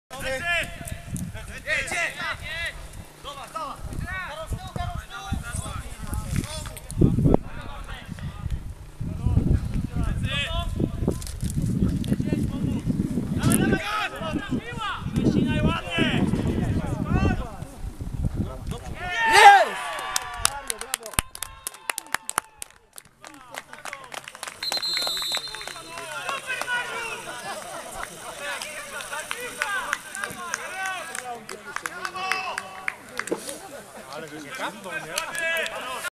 2 0 Alan Reszka po podaniu Mariusza Szpyry
Orzeł Sadowice - Wiwa Goszcz 3:1 (0:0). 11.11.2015  Mecz wrocławskiej Klasy Okręgowej seniorów.  Gol na 2:0 Alana Reszki po podaniu Mariusza Szpyry. Akcję długim podaniem rozpoczął Karol Domagała.